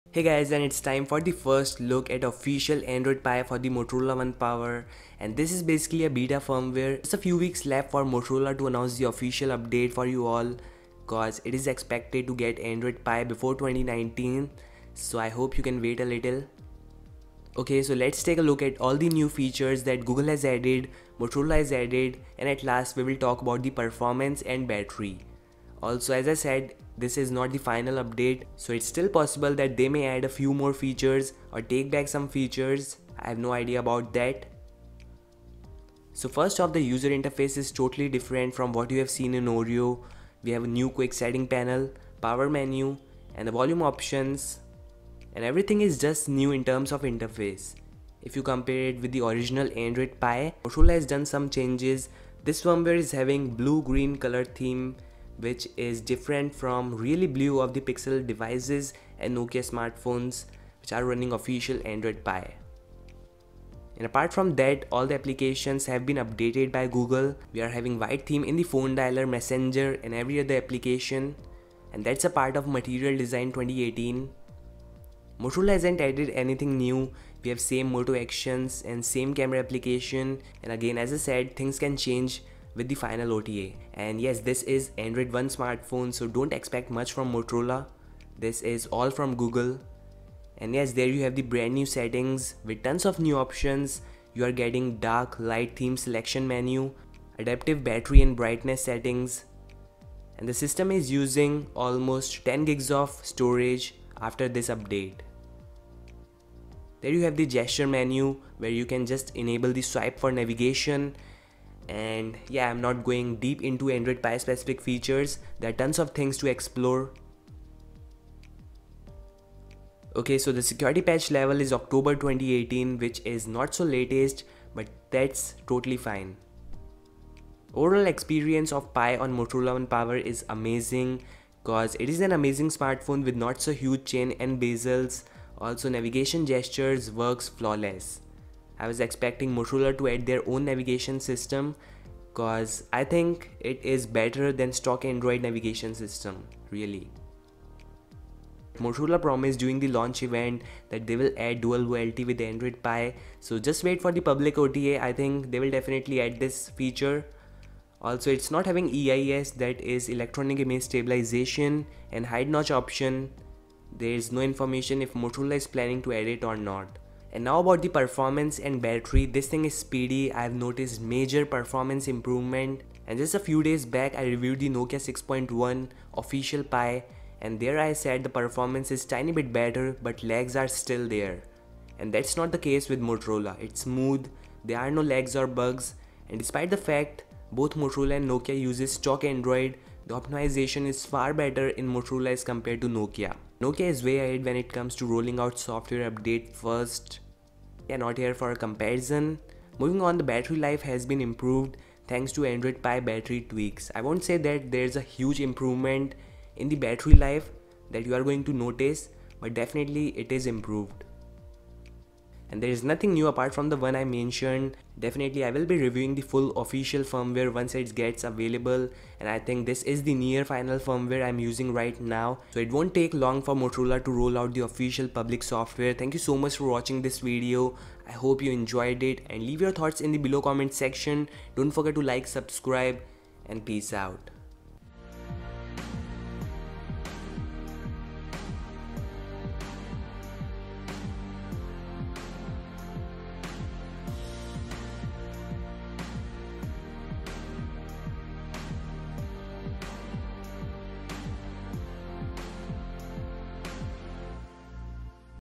Motorola One Power Android 9.0 Pie! New Features/Changes
Motorola One Power Android 9.0 Pie Official Review.. Lets take a look at the Android Pie new features on Motorola One Power.

*Android Pie is now Officially rolling out for the Motorola One Power*

Upgrade system from Android O to Android P
Add LTE+LTE (Dual Volte) feature
Add Panorama feature for camera
Camera improvements
Android security update through 1 November 2018